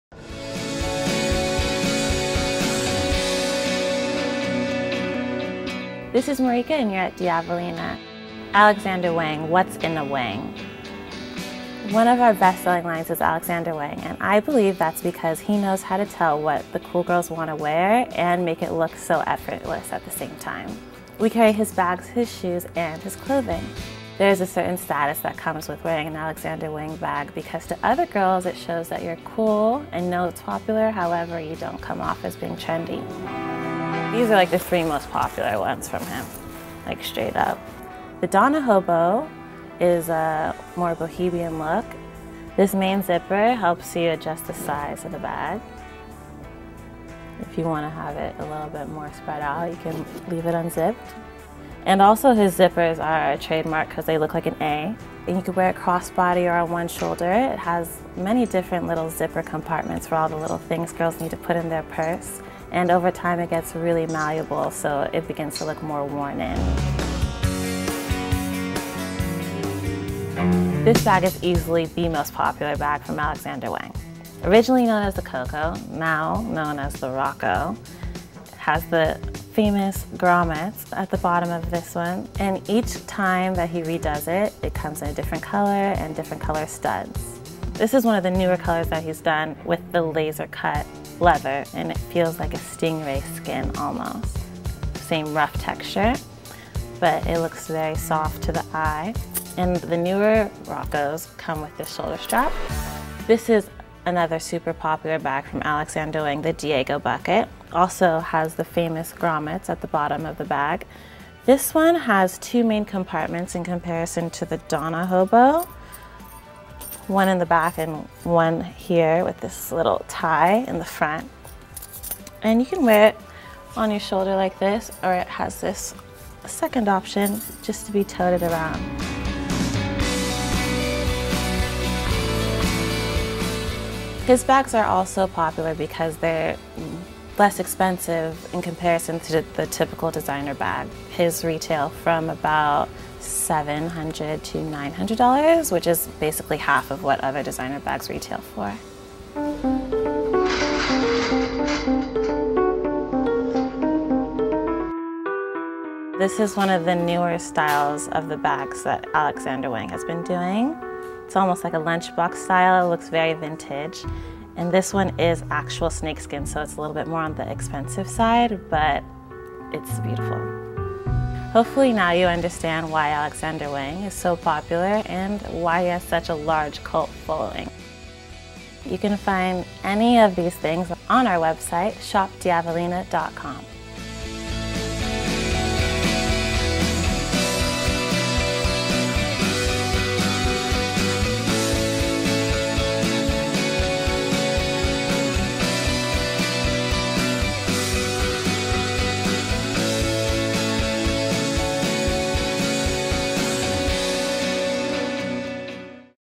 StyleSpotting TV: What's in a Wang?
(StyleSpotting TV) Alexander Wang is on fire over the last couple years and Marika from Diavolina in Los Angeles fills me in on why the cool girls carry his bags - and her shop's Top 3 styles.

Check out the collection at ShopDiavolina.com